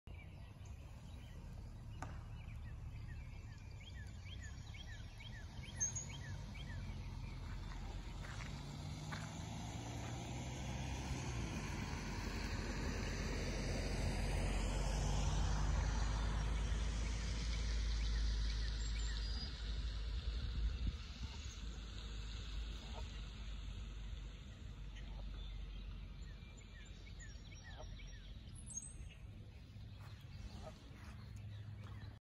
Fish jumps out of the water
A fish jumps out of the water while I was filming the  geese and goslings across the lake.
Zoom in to catch the action. Looks like the fish is coming up to eat!